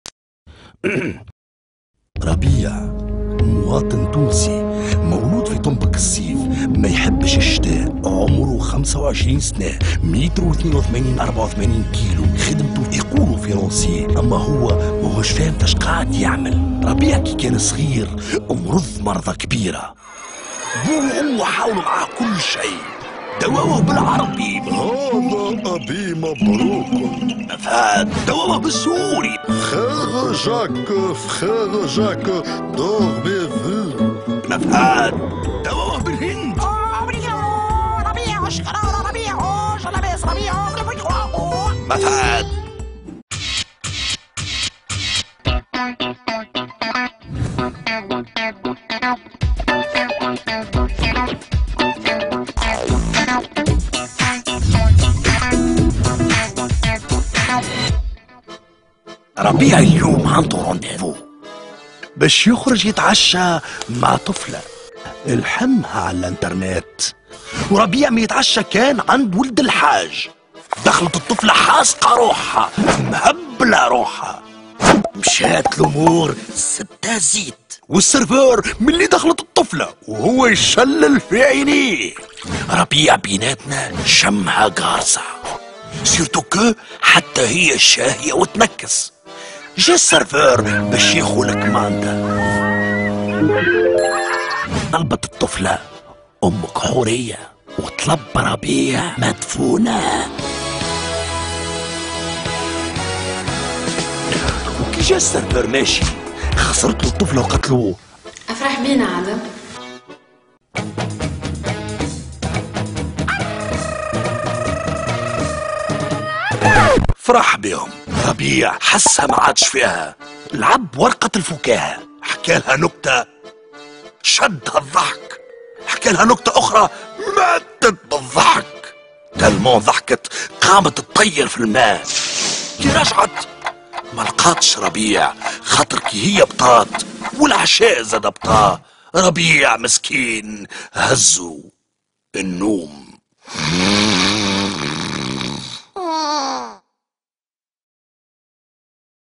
ربيع
شاب تونسي حيّر العلماء بالّلهجة التونسيّة